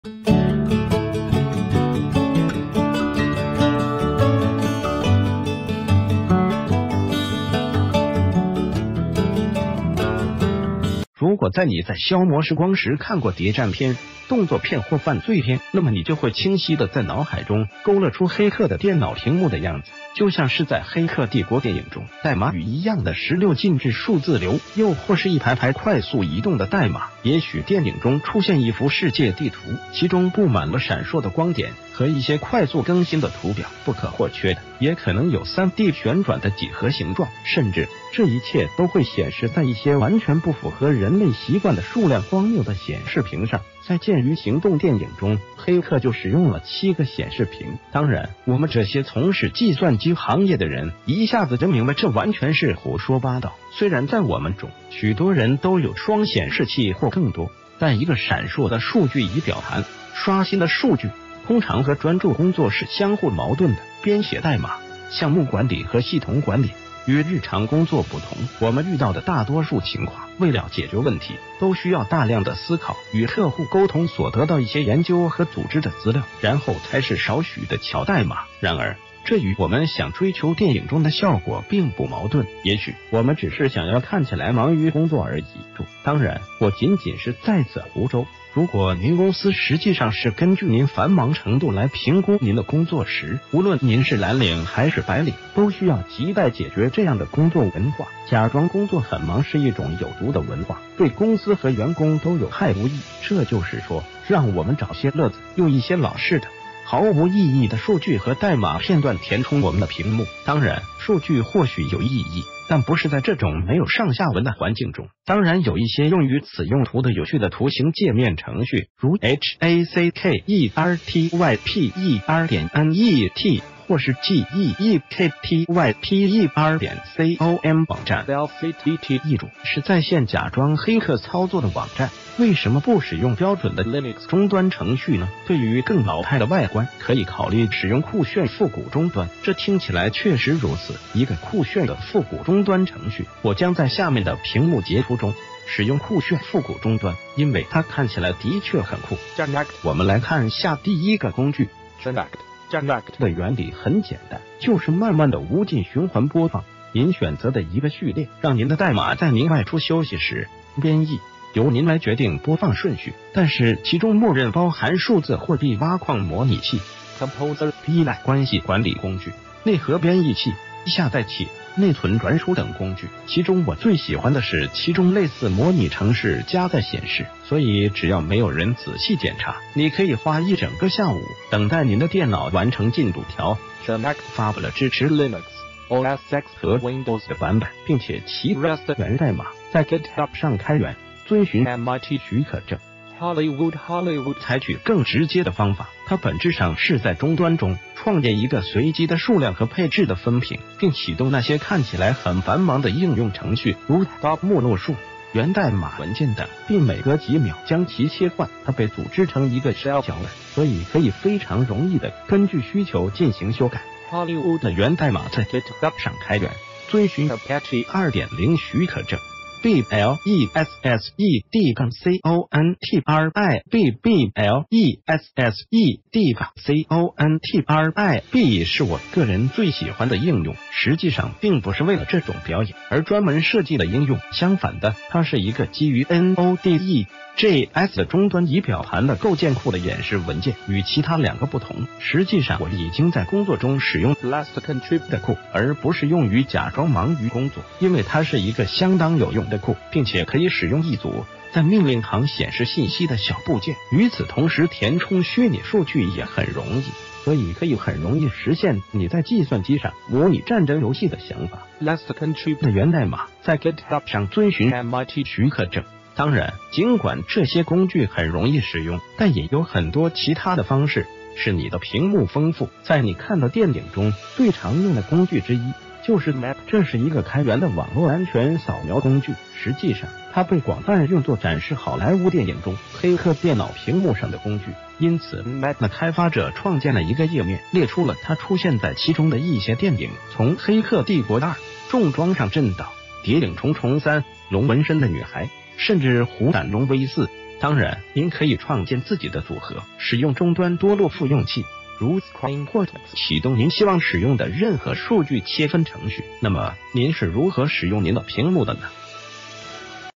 假裝很忙的三個命令行工具
如果在你在消磨時光時看過諜戰片、動作片或犯罪片，那么你就會清晰地在腦海中勾勒出黑客的電腦屏幕的樣子。就像是在《黑客帝國》電影中，代碼雨 一樣的十六進制數字流，又或是一排排快速移動的代碼。
    也許電影中出現一幅世界地圖，其中布滿了閃爍的光點和一些快速更新的圖表。不可或缺的，也可能有 3D 旋轉的幾何形狀。甚至，這一切都會顯示在一些完全不符合人類習慣的數量荒謬的顯示屏上。 在《劍魚行動》電影中黑客就使用了七個顯示屏。
    當然，我們這些從事計算機行業的人一下子就明白這完全是胡說八道。雖然在我們中，許多人都有雙顯示器（或更多），但一個閃爍的數據儀表盤、刷新的數據通常和專注工作是相互矛盾的。編寫代碼、項目管理和系統管理與日常工作不同。我們遇到的大多數情況，為了解決問題，都需要大量的思考，與客戶溝通所得到一些研究和組織的資料，然后才是少許的 敲代碼。
    然而，這與我們想追求電影中的效果并不矛盾，也許，我們只是想要看起來“忙于工作”而已。
    注：當然，我僅僅是在此胡謅。如果您公司實際上是根據您繁忙程度來評估您的工作時，無論您是藍領還是白領，都需要亟待解決這樣的工作文化。假裝工作很忙是一種有毒的文化，對公司和員工都有害無益。
    這就是說，讓我們找些樂子，用一些老式的、毫無意義的數據和代碼片段填充我們的屏幕。（當然，數據或許有意義，但不是在這種沒有上下文的環境中。）當然有一些用于此用途的有趣的圖形界面程序，如 hackertyper.net 或是 GEEKtyper.com 網站（LCTT 譯注：是在線假裝黑客操作的網站），為什么不使用標準的 Linux 終端程序呢？對于更老派的外觀，可以考慮使用 酷炫復古終端，這聽起來確實如此：一個酷炫的復古終端程序。我將在下面的屏幕截圖中使用酷炫復古終端，因為它看起來的確很酷。
    Genact
    我們來看下第一個工具——Genact。Genact 的原理很簡單，就是慢慢地無盡循環播放您選擇的一個序列，讓您的代碼在您外出休息時“編譯”。由您來決定播放順序，但是其中默認包含數字貨幣挖礦模擬器、Composer PHP 依賴關系管理工具、內核編譯器、下載器、內存轉儲等工具。其中我最喜歡的是其中類似《模擬城市》加載顯示。所以只要沒有人仔細檢查，你可以花一整個下午等待您的電腦完成進度條。
    Genact 發布了 支持 Linux、OS X 和 Windows 的版本。并且其 Rust 源代碼 在 GitHub 上開源（遵循 MIT 許可證）。

    Hollywood
    Hollywood 采取更直接的方法。它本質上是在終端中創建一個隨機的數量和配置的分屏，并啟動那些看起來很繁忙的應用程序，如 htop、目錄樹、源代碼文件等，并每隔幾秒將其切換。它被組織成一個 shell 腳本，所以可以非常容易地根據需求進行修改。
    Hollywood的 源代碼 在 GitHub 上開源（遵循 Apache 2.0 許可證）。

    Blessed-contrib
    Blessed-contrib 是我個人最喜歡的應用，實際上并不是為了這種表演而專門設計的應用。相反地，它是一個基于 Node.js 的終端儀表盤的構建庫的演示文件。與其他兩個不同，實際上我已經在工作中使用 Blessed-contrib 的庫，而不是用于假裝忙于工作。因為它是一個相當有用的庫，并且可以使用一組在命令行顯示信息的小部件。與此同時填充虛擬數據也很容易，所以可以很容易實現你在計算機上模擬《戰爭游戲》的想法。
    Blessed-contrib 的源代碼在 GitHub 上（遵循 MIT 許可證）。

    當然，盡管這些工具很容易使用，但也有很多其他的方式使你的屏幕豐富。在你看到電影中最常用的工具之一就是Nmap，這是一個開源的網絡安全掃描工具。實際上，它被廣泛用作展示好萊塢電影中，黑客電腦屏幕上的工具。因此 Nmap 的開發者創建了一個 頁面，列出了它出現在其中的一些電影，從《黑客帝國 2：重裝上陣》到《諜影重重3》、《龍紋身的女孩》，甚至《虎膽龍威 4》。
    當然，您可以創建自己的組合，使用終端多路復用器（如 screen 或 tmux）啟動您希望使用的任何數據切分程序。
    那么，您是如何使用您的屏幕的呢？